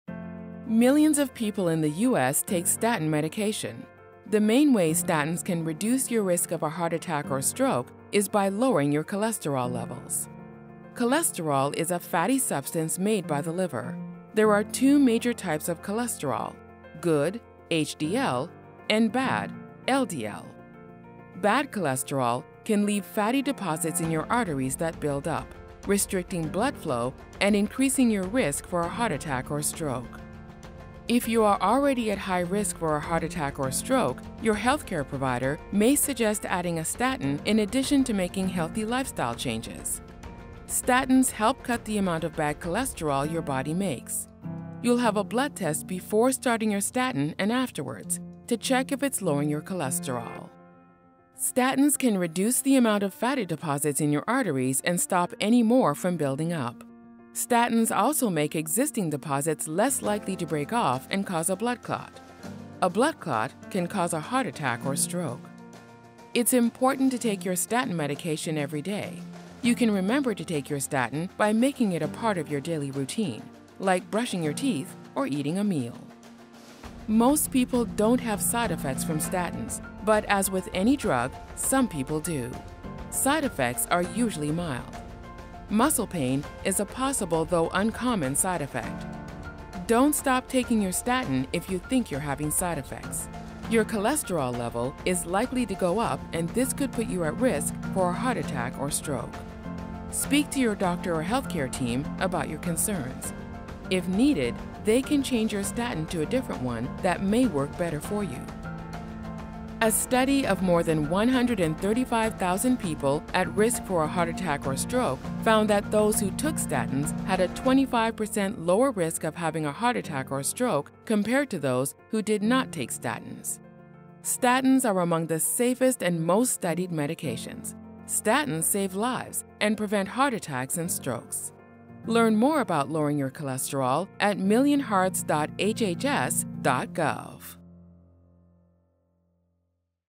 How do statins prevent heart attacks and strokes?
Statins are a type of medication used to lower the level of bad cholesterol in the blood and reduce build-up in arteries that can cause a heart attack or stroke. This short animated video explains the importance of statins, how they work, and why your doctor may prescribe them.